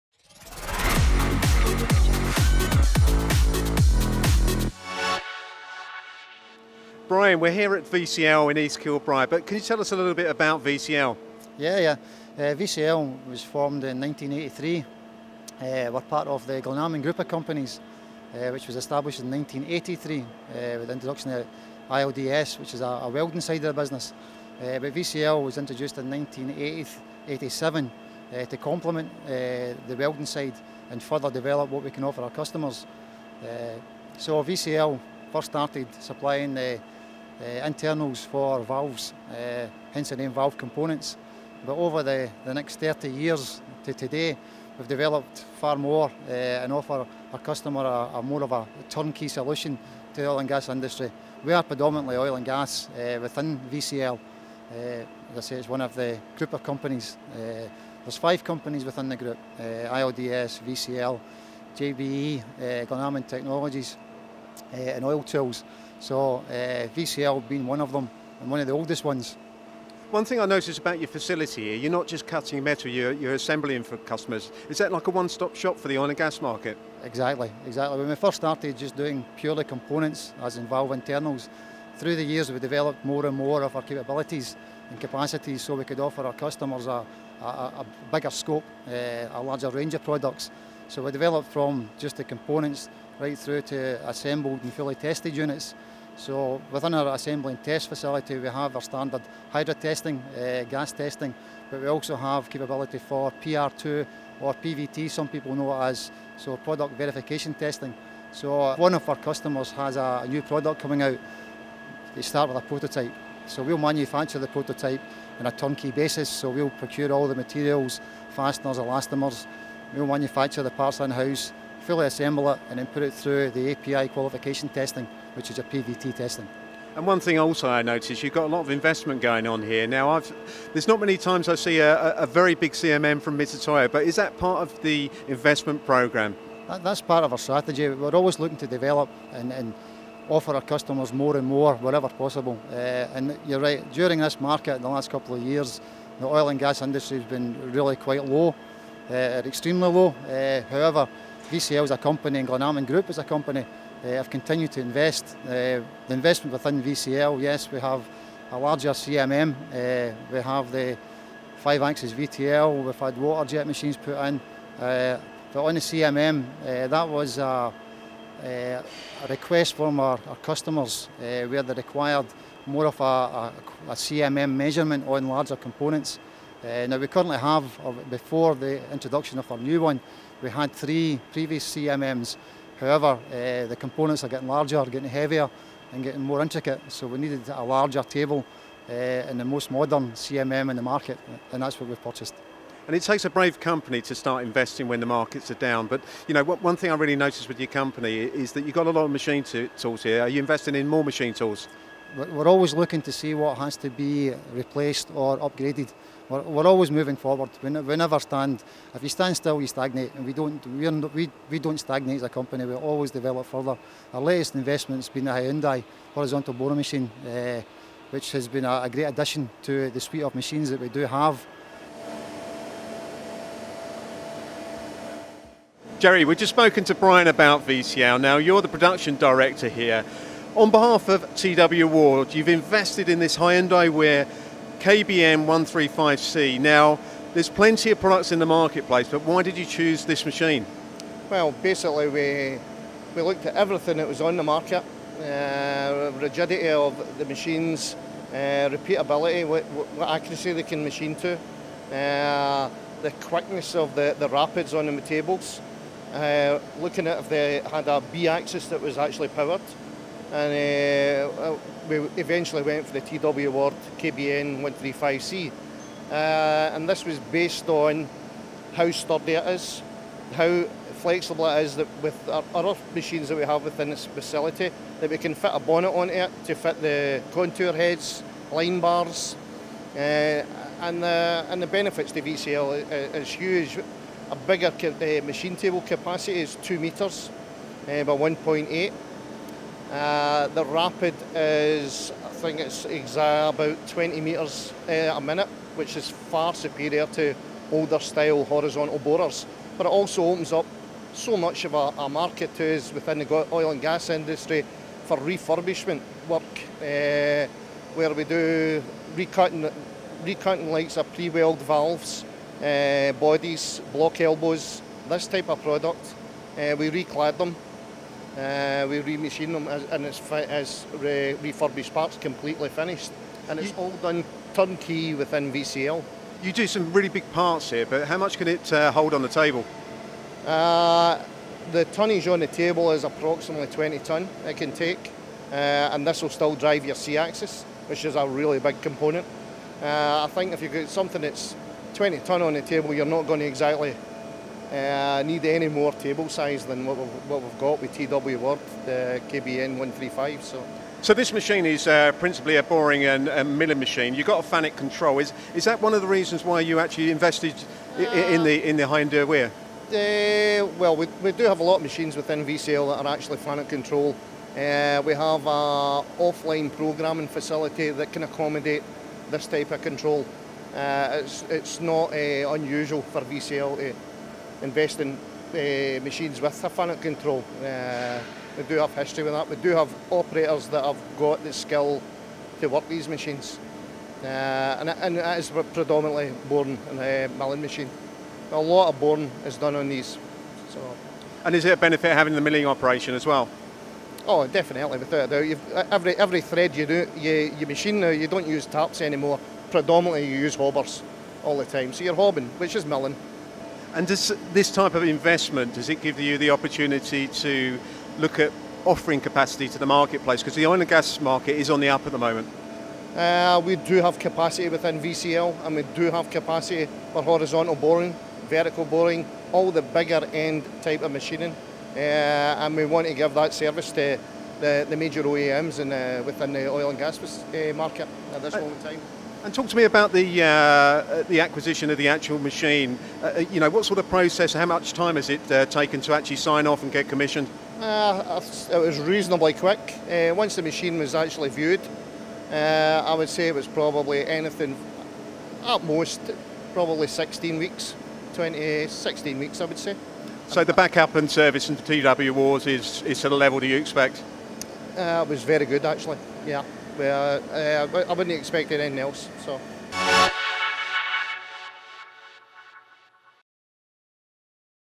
T W Wards support VCL and the Oil & Gas sector
MTD Visit VCL in East Kilbride to view their new, massive capacity Hyundai Wia KBN135C Horizontal Borer recently supplied & installed by T W Ward CNC Machinery Ltd of Sheffield. This rugged cross bed design machine features 15T table loading and axes travel of 3m in X; 2.5m in Y; 1.6m in Z and quill travel of 700mm. The Hyundai Wia KBN horizontal borers feature high accuracy linear scales as standard and this combined with the capacity and robustness provide a machine suited to part manufacture for most industry applications. Contact Ward CNC for further details and to discuss the full Hyundai Wia range.